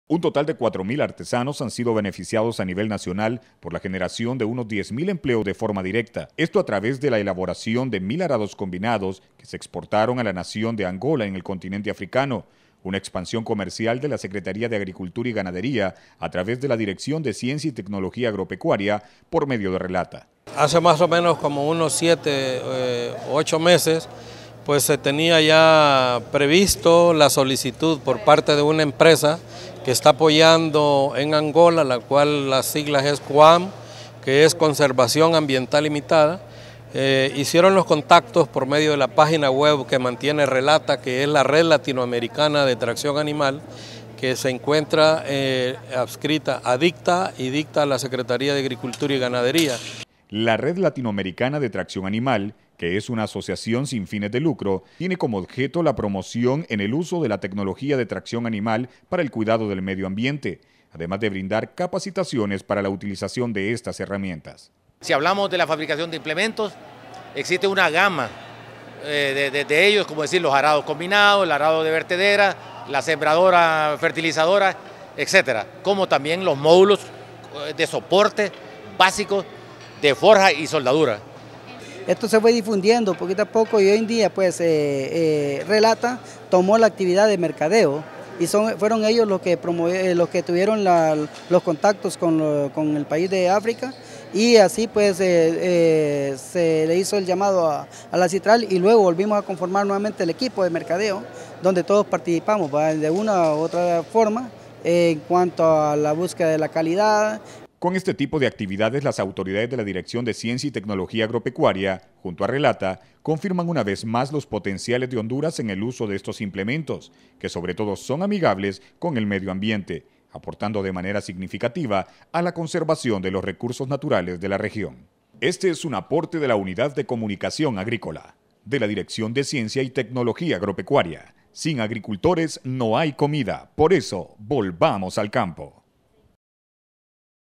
VTR entrega arado convinado RELATA DICTA
La Red Latinoamericana de Tracción Animal, junto a la Dirección de Ciencia y Tecnología Agropecuaria, en Honduras, fomentan la exportación de arados combinados a otros países.